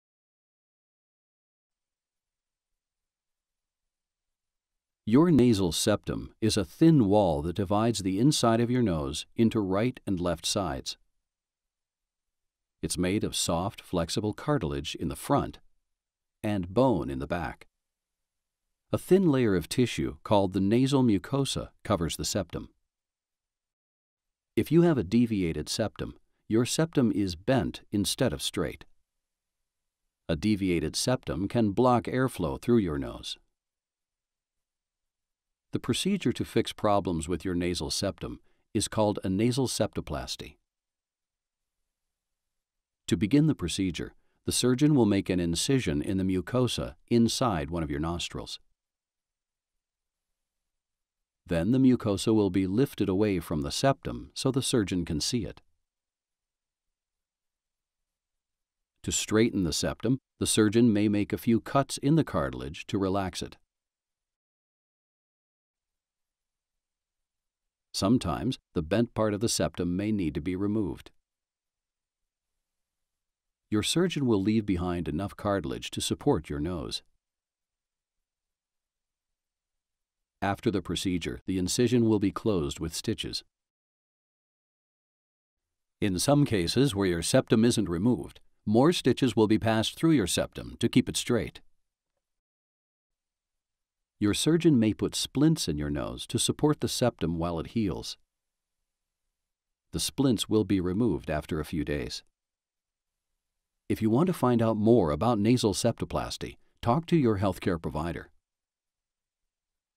Nasal Septoplasty – CIMS Hospital
This video will help us to know nasal septoplasty. A thin layer of tissue called the nasal mucosa covers the septum. Your nasal septum is a thin wall that divides the inside of your nose into right and left sides. It's made of soft, flexible cartilage in the front and bone in the back.